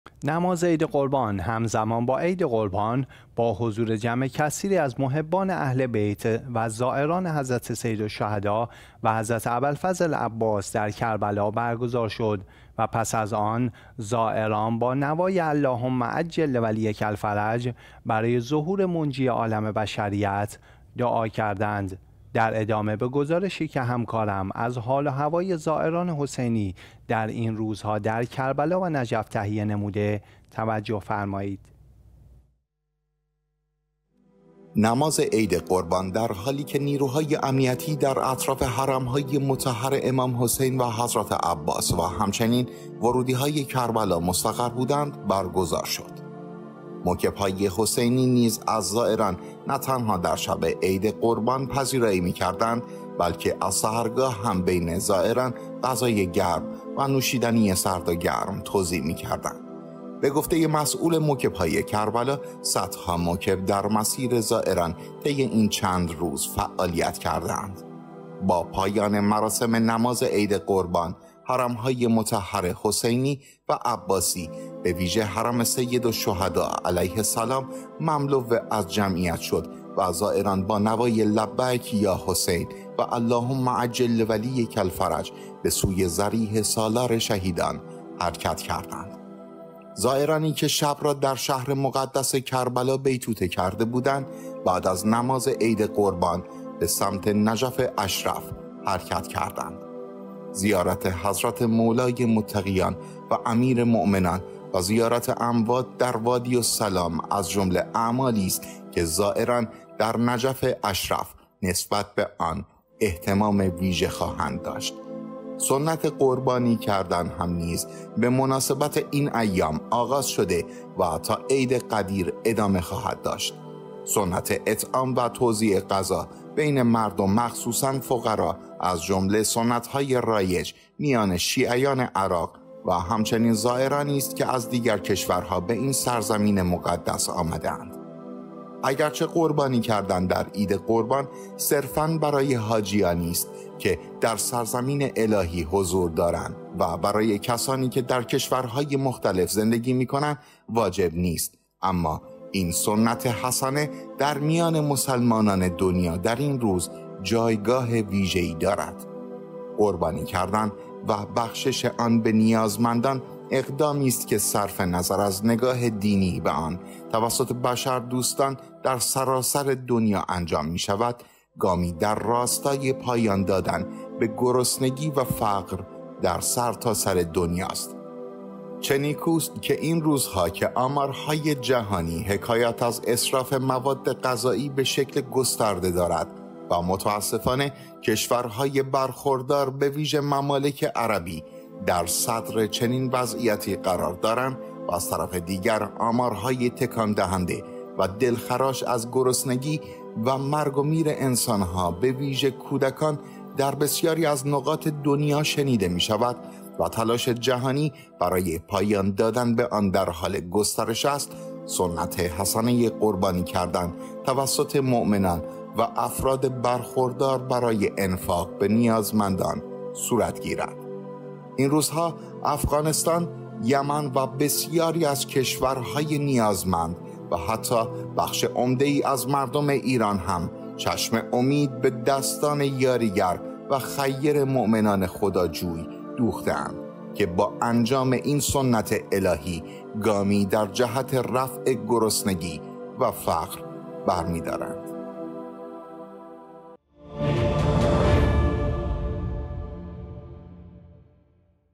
نگاهی به حال و هوای حرم‌های مطهر در شهر مقدس کربلا و نجف اشرف
نماز عید قربان همزمان با عید قربان با حضور جمع کثیری از محبان اهل بیت زائران حضرت سیدالشهداء و حضرت اباالفضل العباس در کربلا برگزار شد و پس از آن زائران با نوای «اللهم عجل لولیک الفرج» برای ظهور منجی عالم بشریت دعا کردند.

 نماز عید قربان در حالی که نیروهای امنیتی در اطراف حرم‌های مطهر امام حسین و حضرت عباس و همچنین ورودی‌های کربلا مستقر بودند، برگزار شد.

موکب‌های حسینی نیز از زائران نه تنها در شب عید قربان پذیرایی می‌کردند بلکه از سحرگاه هم بین زائران غذای گرم و نوشیدنی سرد و گرم توزیع می‌کردند، به گفته مسؤول موکب‌های کربلا، صدها موکب در مسیر زائران طی این چند روز  فعالیت کرده‌اند.

با پایان مراسم نماز عید قربان، حرم‌های مطهر حسینی و عباسی به ویژه حرم سیدالشهداء علیه‌السلام مملو از جمعیت شد و زائران با نوای لبیک یا حسین و اللهم عجل لولیک الفرج به سوی ضریح سالار شهیدان حرکت کردند.

زائرانی که شب را در شهر مقدس کربلا بیتوته کرده بودند بعد از نماز عید قربان به سمت نجف اشرف حرکت کردند.

زیارت حضرت مولای متقیان و امیرمومنان و زیارت اموات در وادی السلام از جمله اعمالی است که زائران در نجف اشرف نسبت به آن اهتمام ویژه خواهند داشت.

سنت قربانی کردن هم نیز به مناسبت این ایام، آغاز شده و تا عید غدیر ادامه خواهد داشت.

سنت اطعام و توزیع غذا بین مردم مخصوصا فقرا از جمله سنت‌های رایج میان شیعیان عراق و هم چنین زائرانی است که از دیگر کشورها به این سرزمین مقدس آمده‌اند.

اگر چه قربانی کردن در عید قربان صرفا برای حاجیانی که در سرزمین الهی حضور دارند، واجب است و برای کسانی که در کشورهای مختلف زندگی می کنند، واجب نیست، اما این سنت حسنه در میان مسلمانان دنیا در این روز، جایگاه ویژه ای دارد.

قربانی کردن و بخشش آن به نیازمندان اقدامی است که صرف نظر از نگاه دینی به آن توسط بشردوستان در سراسر دنیا انجام می‌شود، گامی در راستای پایان دادن به گرسنگی و فقر در سر تا سر دنیاست

چه نیکوست که این روزها که آمارهای جهانی حکایت از اسراف مواد غذایی به شکل گسترده دارد و متاسفانه کشورهای برخوردار به ویژه ممالک عربی در صدر چنین وضعیتی قرار دارند و از طرف دیگر آمارهای تکان دهنده و دلخراش از گرسنگی و مرگ و میر انسان‌ها به ویژه کودکان در بسیاری از نقاط دنیا شنیده می‌شود و تلاش جهانی برای پایان دادن به آن در حال گسترش است، سنت حسنه قربانی کردن توسط مومنان و افراد برخوردار برای انفاق به نیازمندان صورت گیرد.

این روزها افغانستان، یمن و بسیاری از کشورهای نیازمند و حتی بخش عمده ای از مردم ایران هم چشم امید به دستان یاریگر و خیَر مومنان خداجویی دوخته اند که با انجام این سنت الهی ، گامی در جهت رفع گرسنگی و فقر برمی‌دارند